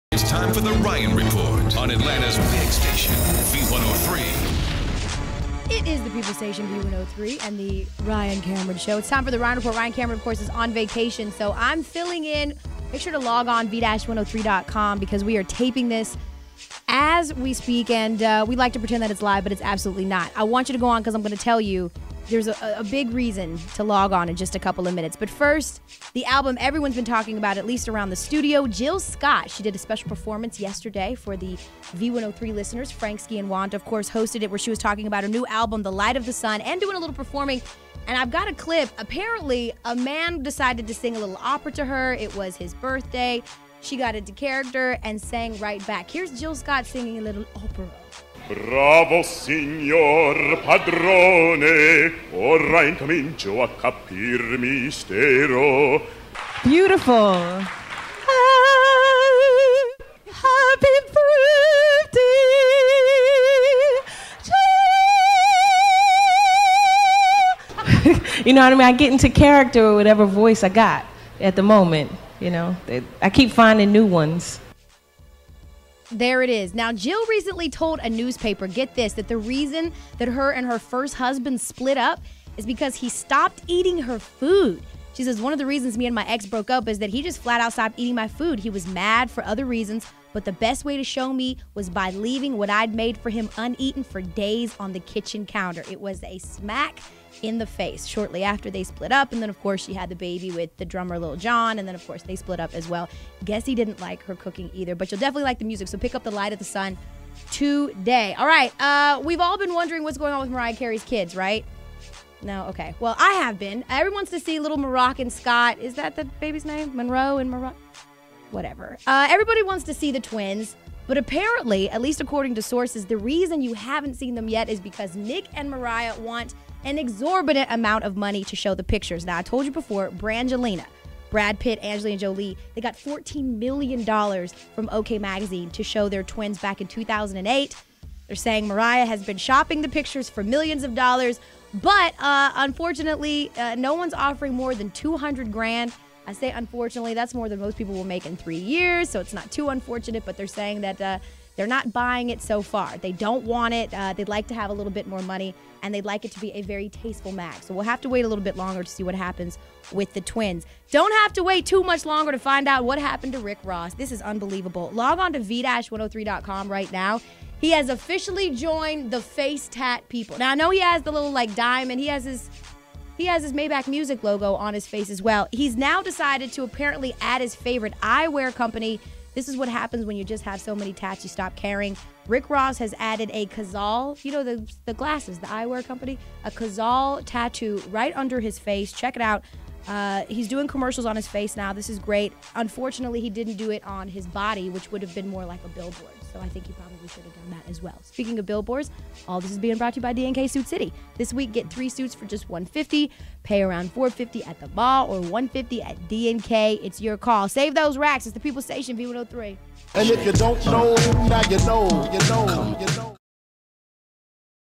The Ryan Report: 06/21/11
Today on the report:

• Jill Scott singing opera?
• Guess how much Nick and Mariah want for pix of their kids?
• Rick Ross joins the face tat club